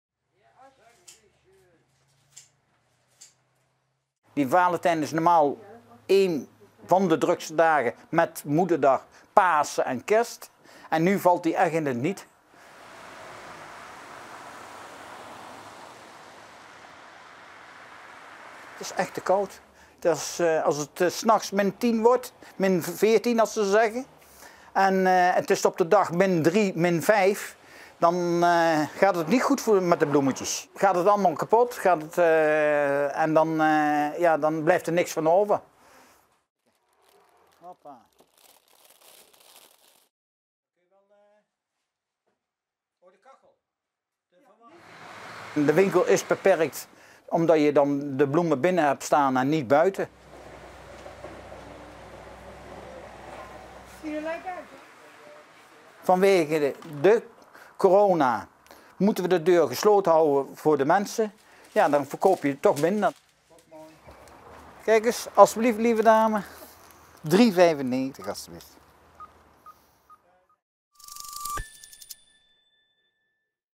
Bloemisten verkopen nauwelijks Valentijnsrozen vanwege de kou
Duiven - Het winterweer is leuk, maar niet voor veel bloemisten. Die verkopen vanwege de kou in combinatie met de coronaregels veel minder boeketten dan voorgaande jaren. En dat is een flinke domper, want normaal is de periode voor Valentijnsdag een van de drukste van het jaar.